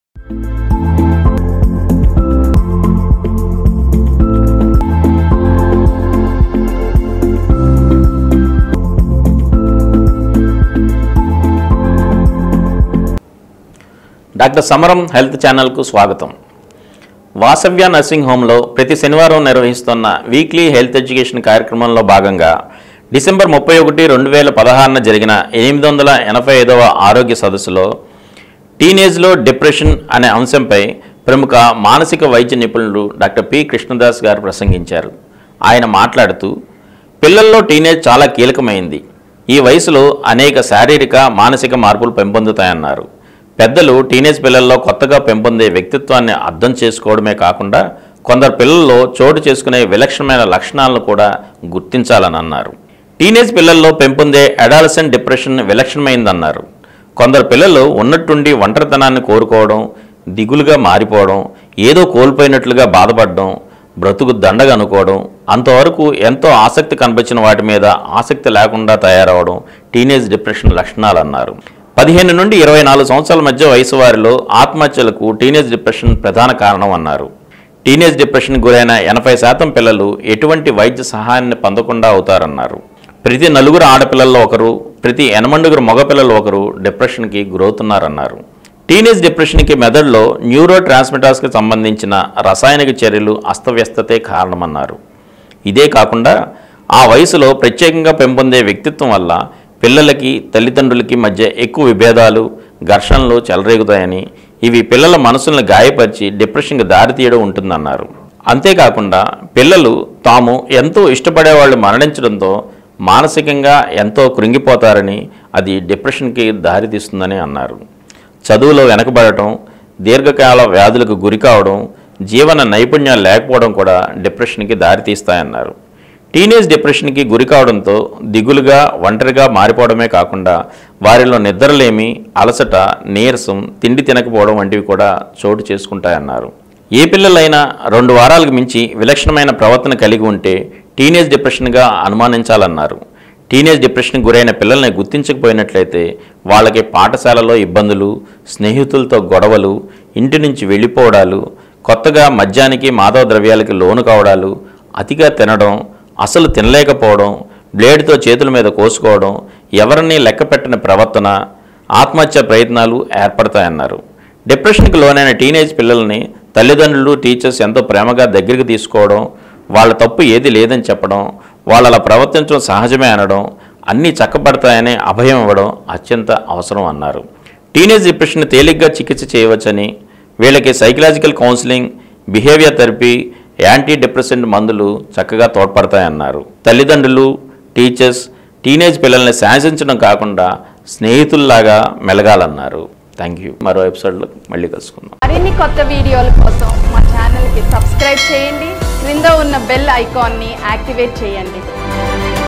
టీనేజీలో డిప్రెషన్ Dr Samaram's Vasavya Health Care (885 prog)on Depression in Teenage
Dr Samaram's Vasavya Health Care (885 prog) by Dr P Krishna Das on Depression in Teenage. Read by Ravi Kumar Dondapati in Dr Samaram Health Channel.